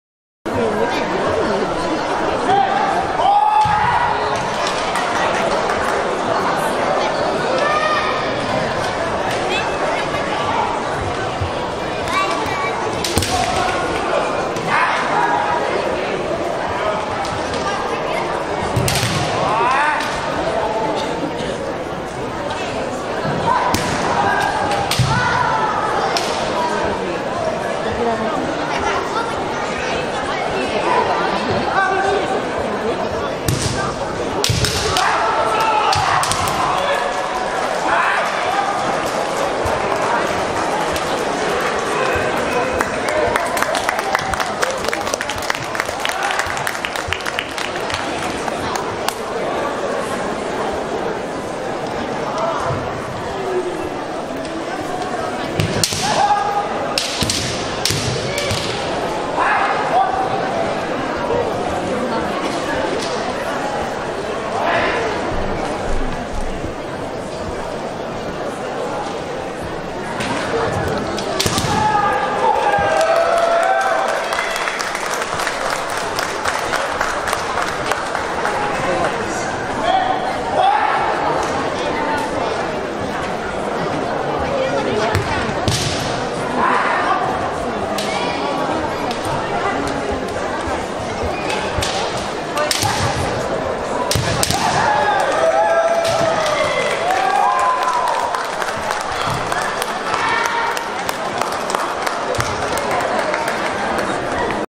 8th AUSKF JR. National Kendo Championships: Boy’s 16-18 Q.Finals 4
2018  8th AUSKF Open National Jr. Kendo Tournament took place in Huntington Beach, California.  Close to 300 competitors from all over the USA.  The Tournament had both Individual and Team Divisions.

The Boy’s Individual16-18 Division 4th Quarter Finals match between WKF and SCKO